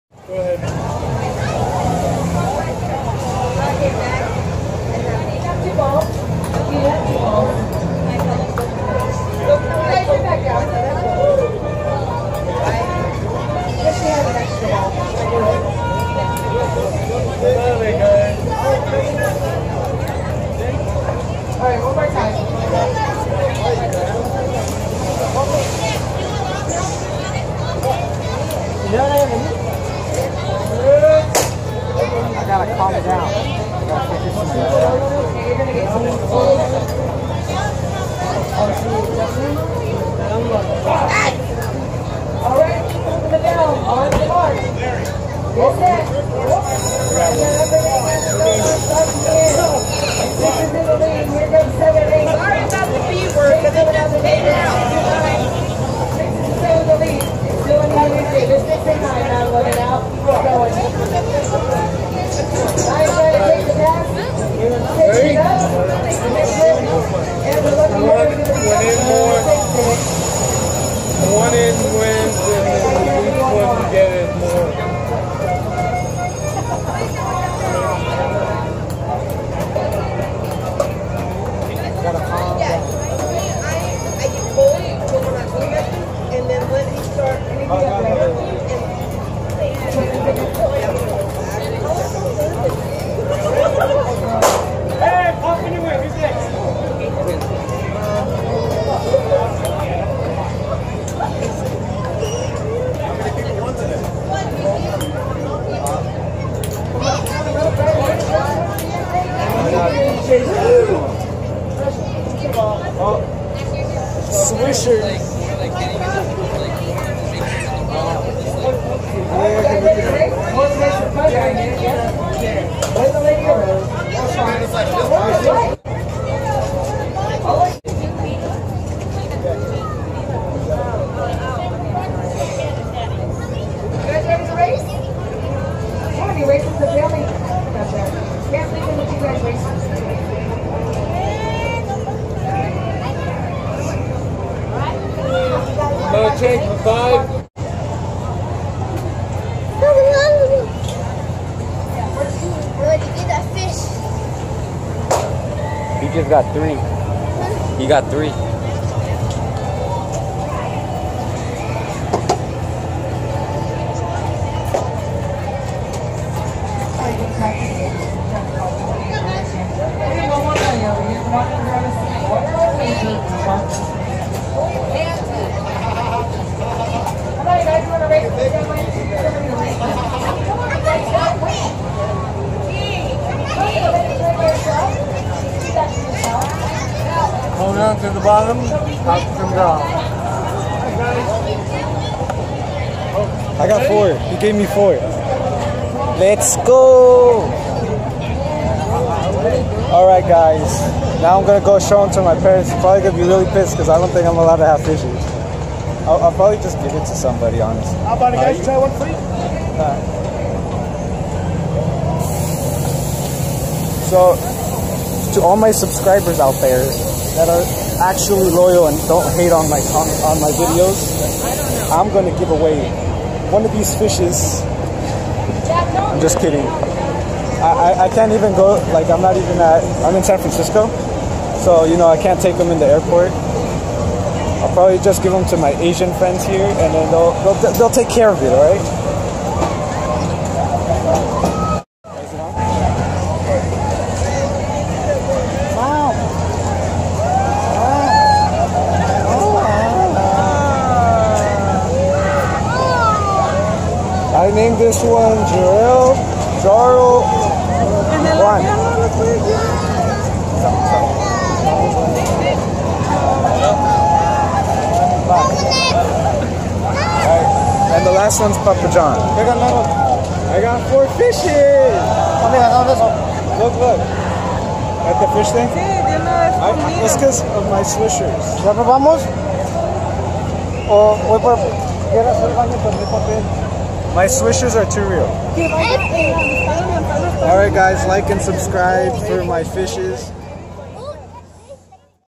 WINNING TONS OF FISH AT THE CARNIVAL!
I woan alot of fish a carnival game